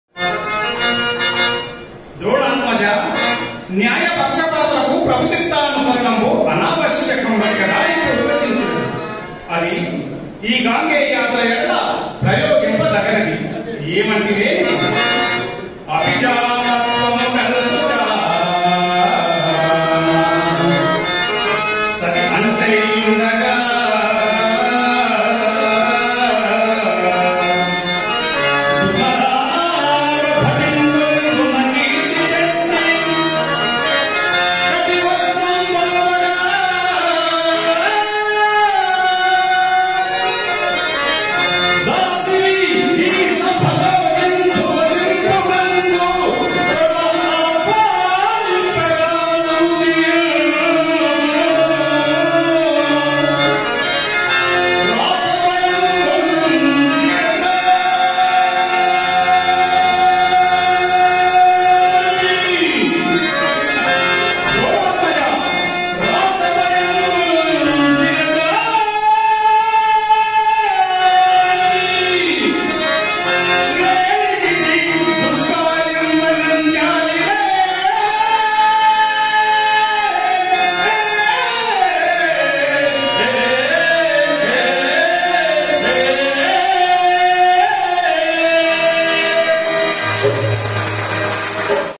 ప్రముఖ రూపశిల్పి వెంకటస్వామి పాడిన పద్యం ll నామా చంద్రబాబు నాయుడు బర్త్ డే కార్యక్రమం లో..
ప్రముఖ హార్మోనిష్టు డా. నామా చంద్రబాబు నాయుడు గారి జన్మదిన ( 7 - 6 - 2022 ) సందర్భంగా శిష్యులు మరియు మిత్రులు సమక్షంలో భారత్ వికాస్ పరిషత్, కూకట్ పల్లి లో జరిగిన పద్య పఠనం కార్యక్రమం.